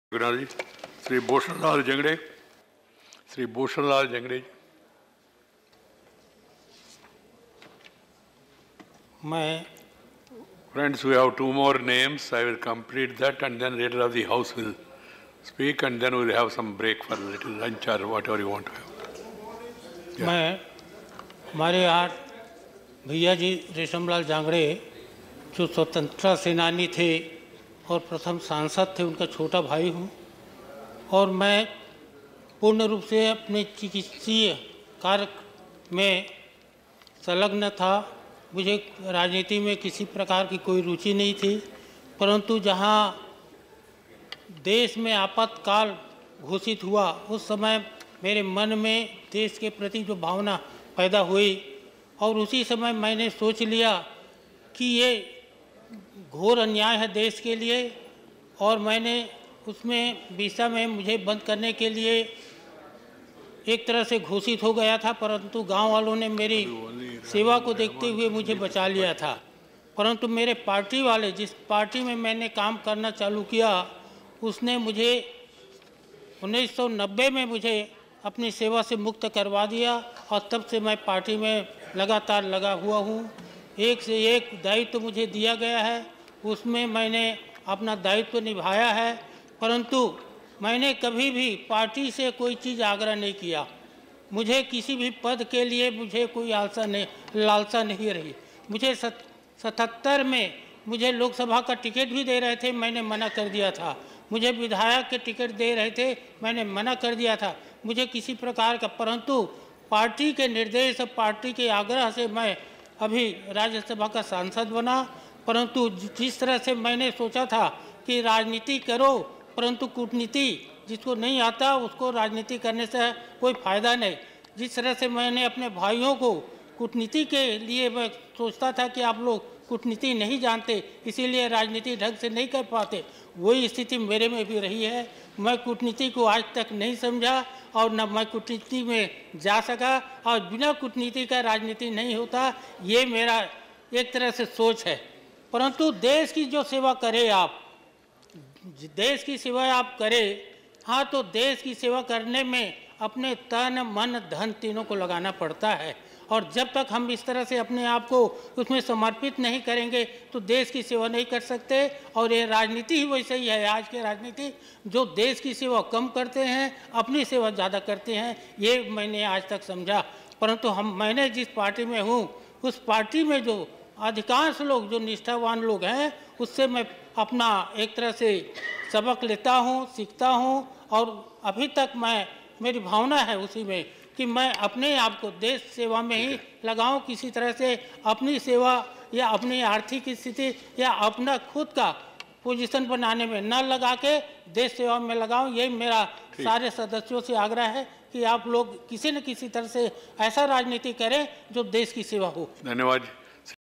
Dr. Bhushan Lal Jangde's speech | Farewell to retiring Rajya Sabha members
Rajya Sabha bids farewell to retiring members of the House, March 28, 2018